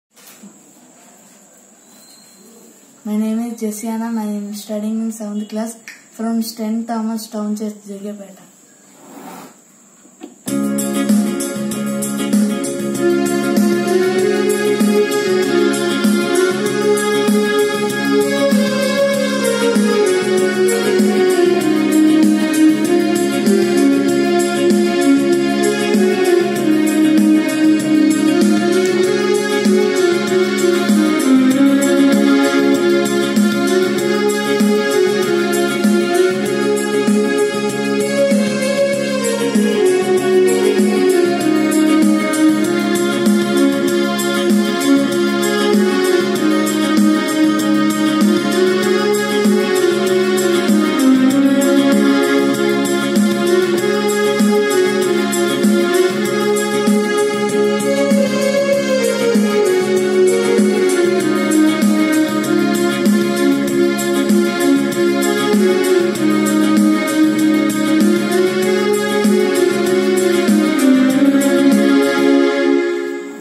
Eco Video Competition | Solo KeyBoard Playing | Regional Level | AndhraPradesh
Church of South India
Department of Ecological Concerns & Department of Christian Education
Eco Video Competition
Solo KeyBoard Playing
Regional Level
AndhraPradesh